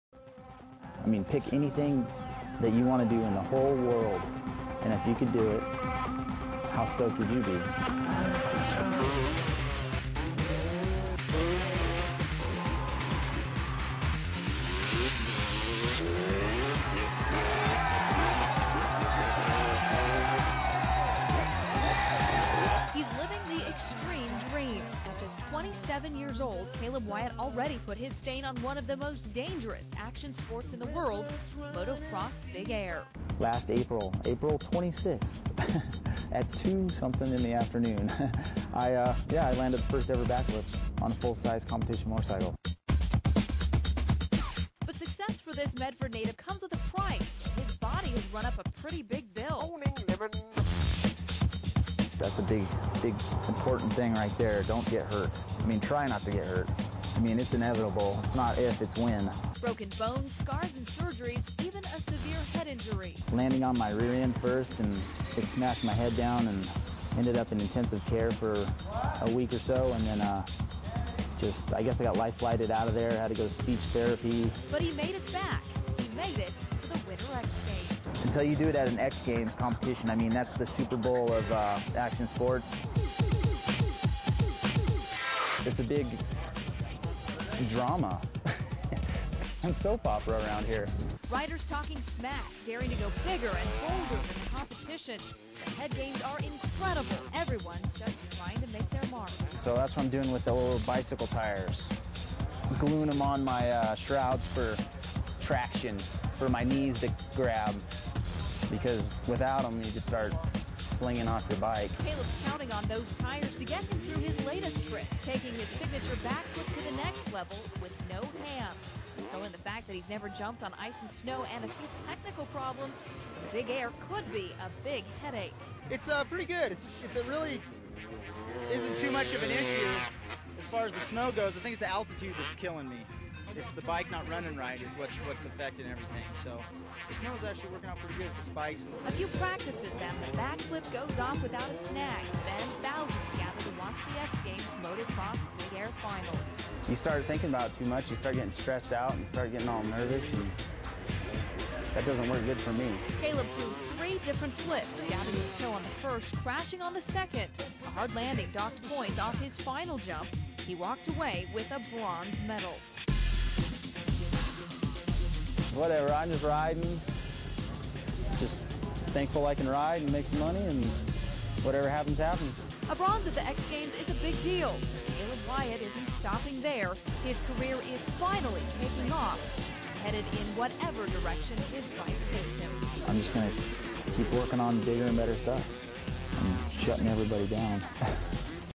Winter X Games - Moto X
A story on Caleb Wyatt in 2002 Winter X Games